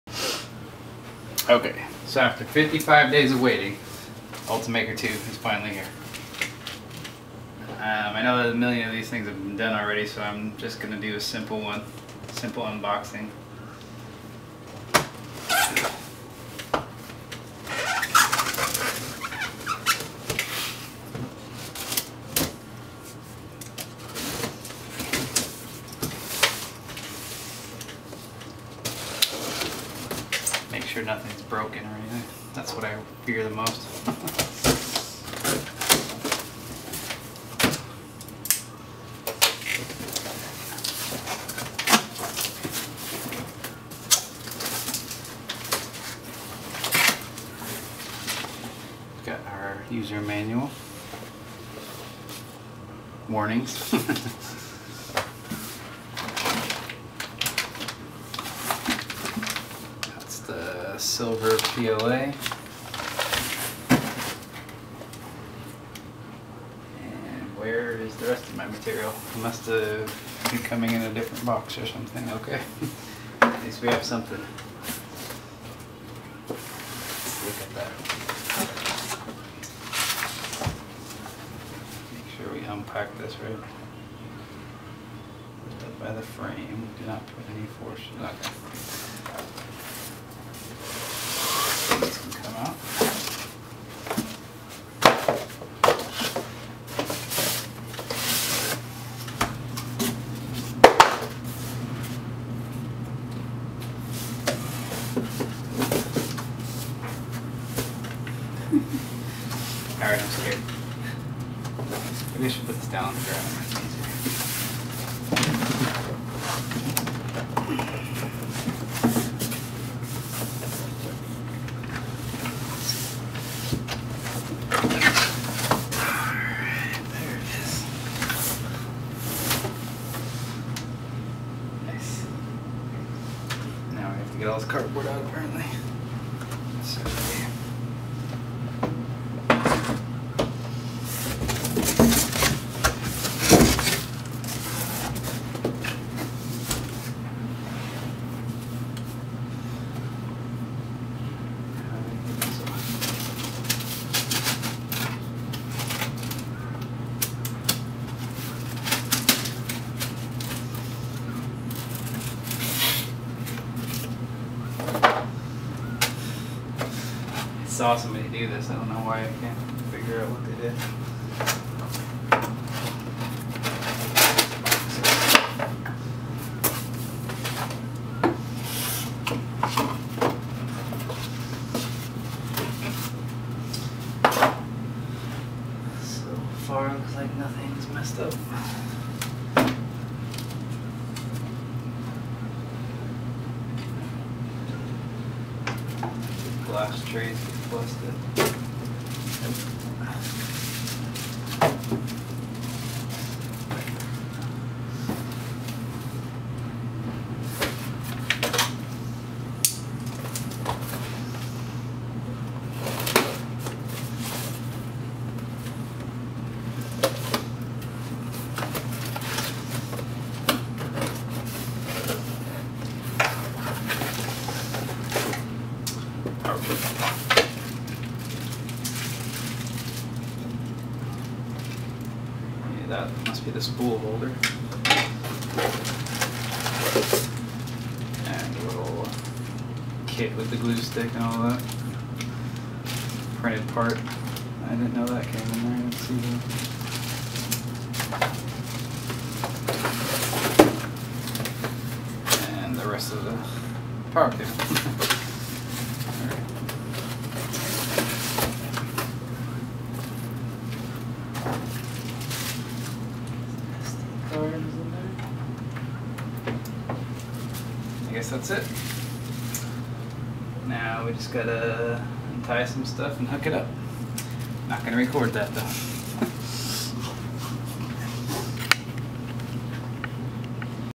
Unboxing the Ultimaker 2 3d printer
This is the first and only unboxing video I have done.  It's very rough and unedited, but I could not help but share with everyone.  I received my other PLA and ABS material in the mail a couple of days after this.